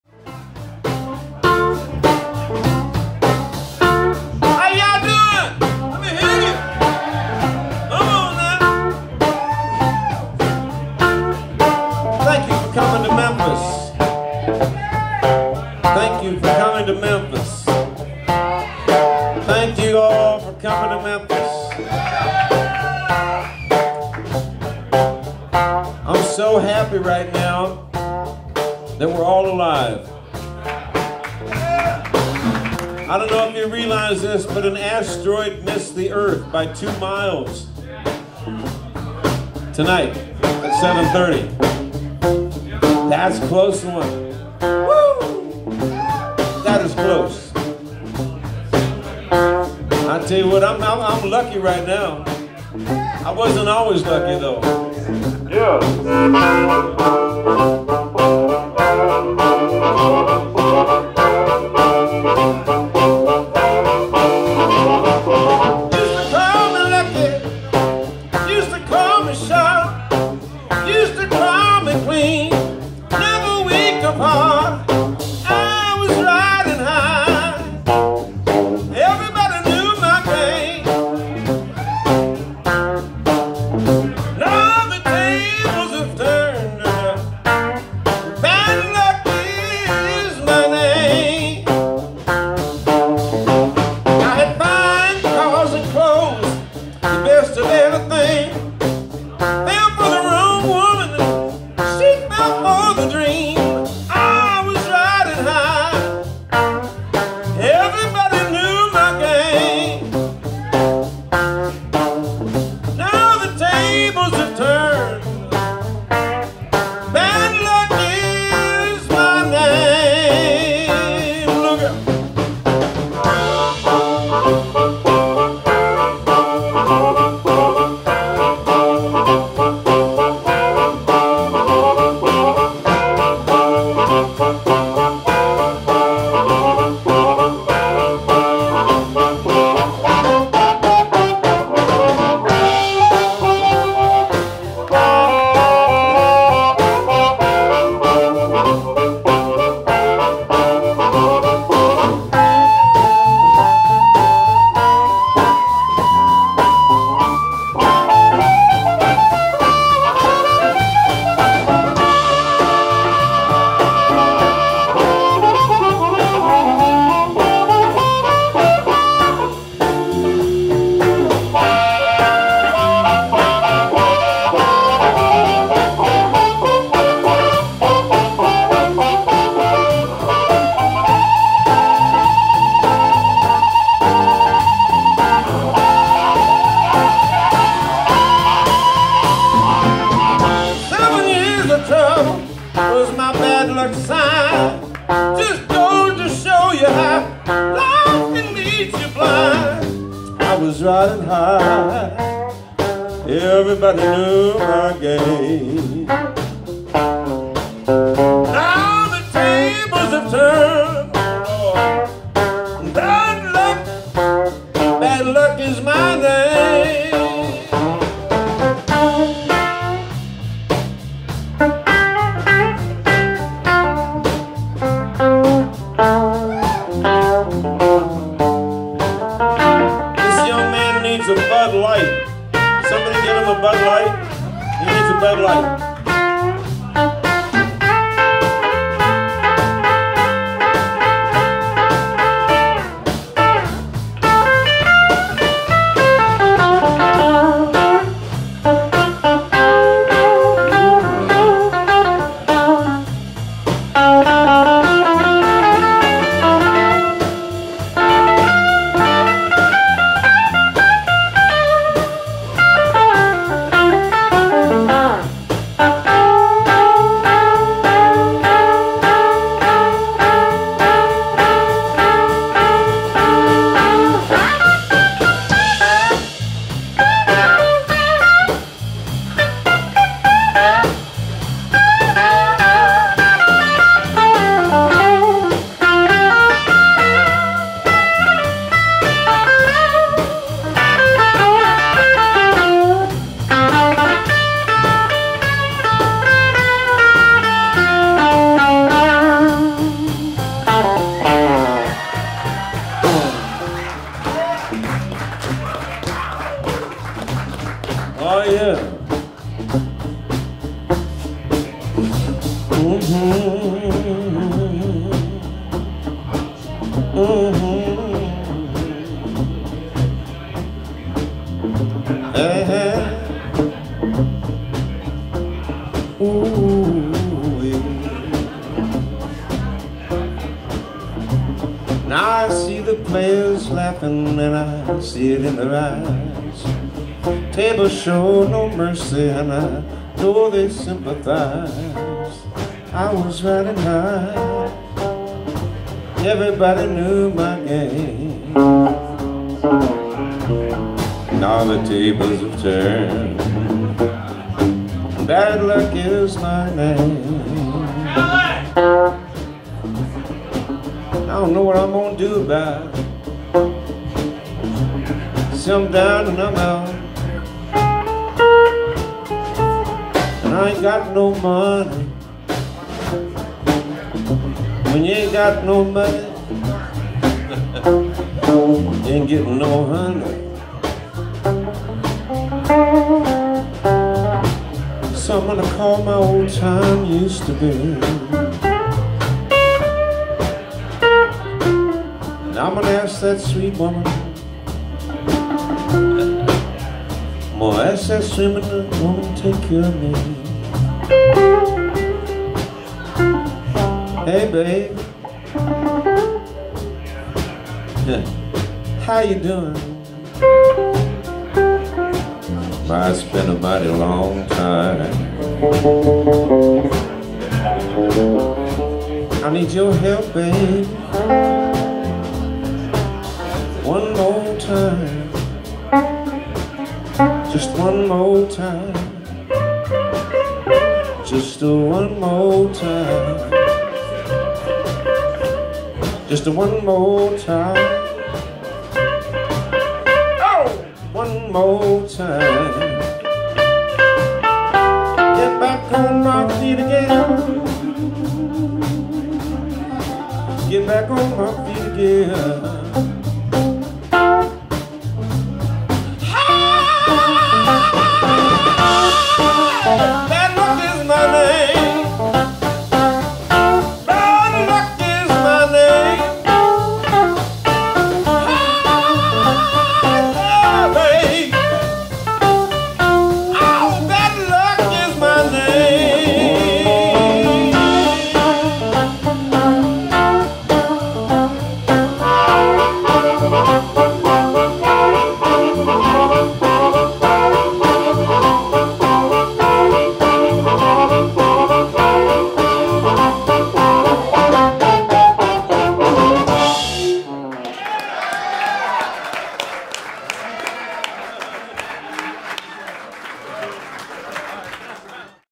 John Nemeth at Memphis Pro Jam 2023
John Nemeth on harp, playing through the Delta V6 Club amp, using a bullet mic with Heumann element.  The other players are Jad Tariq on guitar, Andrew McNeill on drums, and Benton Parker on bass.  The venue is Blues Hall Juke Joint on Beale Street in Memphis during the International Blues Challenge,  January 26, 2023.